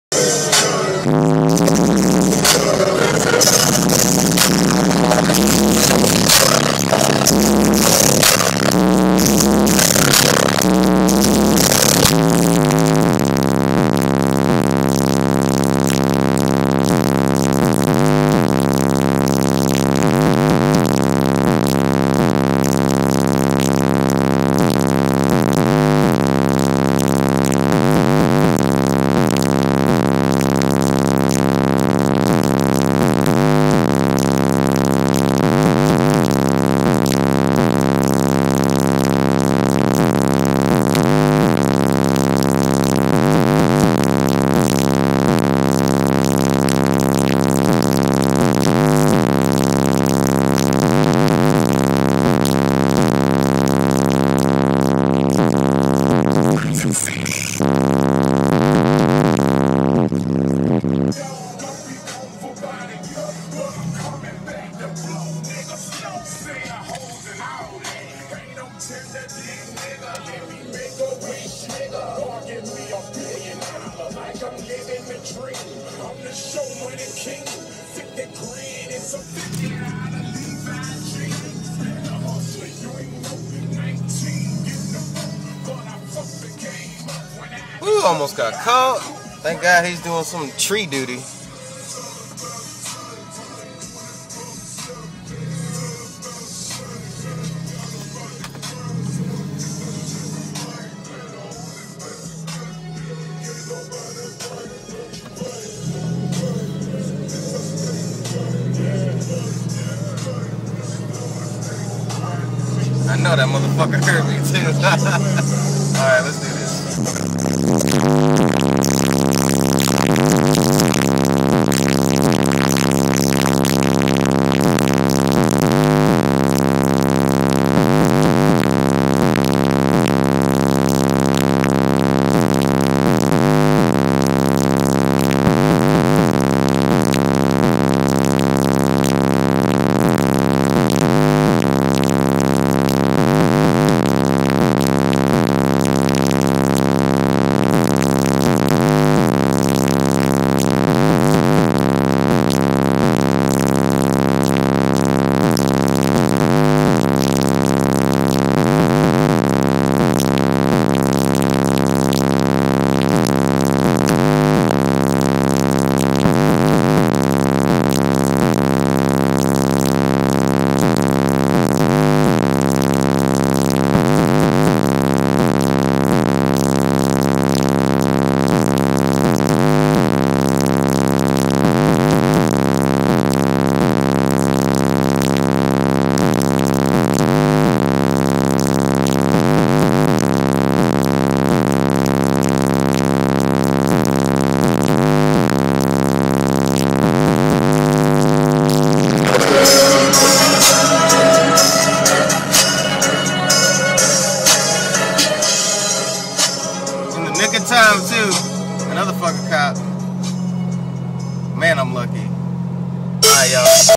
4 IA DP 18's/ 2 RD D9's - Ride Along!
Just woke up, got some breakfast and wanted to hear some bass this morning lol. iPhone camera sucks, there was A LOT  more flex going on, it can't record it for some reason, oh well. Enjoy!